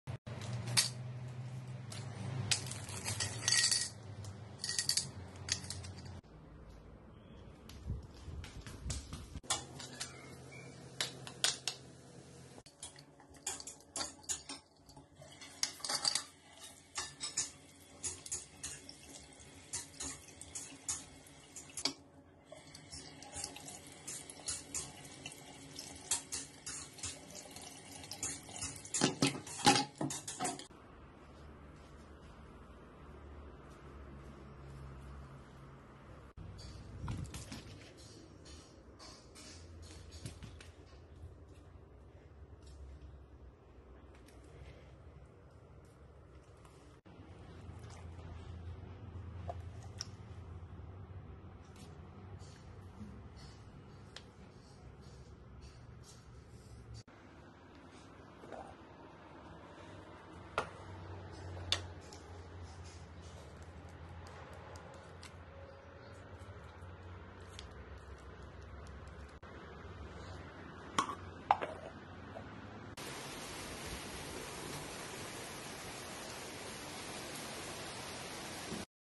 chaotic daiso buy
clearly had too much fun with these water guns, but hey, cheap thrills aren't always so bad.